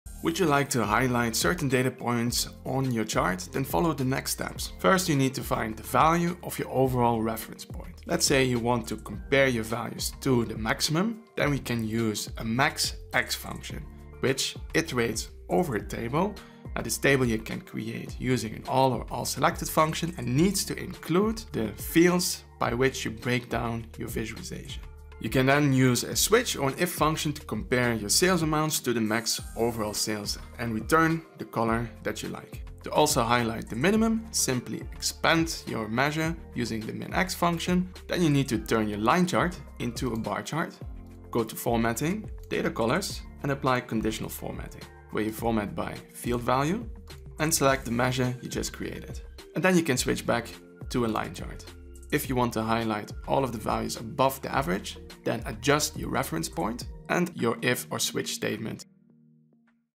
HIGHLIGHT DATAPOINTS in Power BI
Learn how to calculate minimum, maximum and highlight your chosen datapoints in Power BI in 1 Minute.

Do you want to learn Power BI desktop? Are you a beginner?

👇 CHECK THIS OUT! 👇
💻 My gear
📚 Power BI books MUST READ!
💡 General books I recommend
🏄 Stuff I use daily